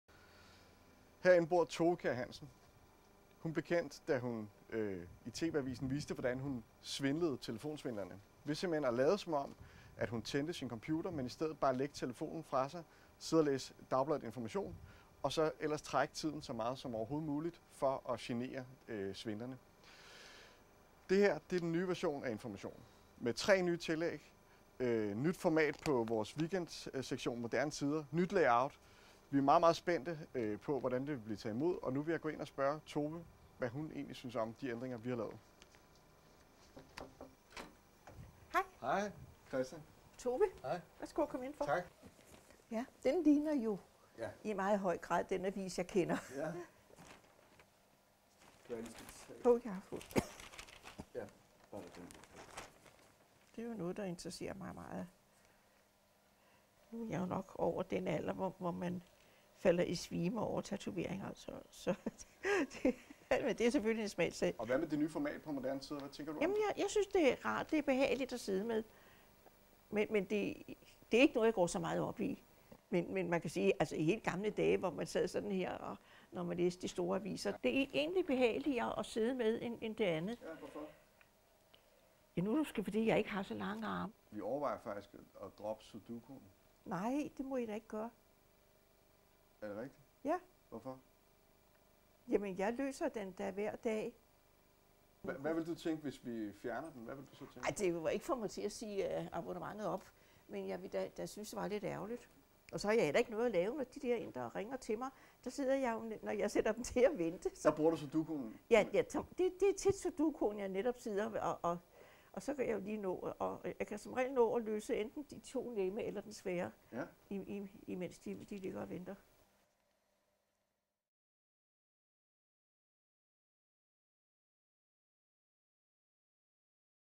Velkommen til ny Information
Information er skabt i kampen for det frie ord, da ordet ikke længere var frit i dette land. Vi er skabt i ambitionen om kompromisløs kritik over for alle, der har myndighed til at bestemme over os andre.

De ændringer, vi med en vis stolthed præsenterer i de kommende dage, er foretaget i bestræbelsen på at videreføre og videreudvikle alt det, der til alle tider gør Information til et særligt mediehus.
God fornøjelse med en ny Information baseret på de gamle dyder.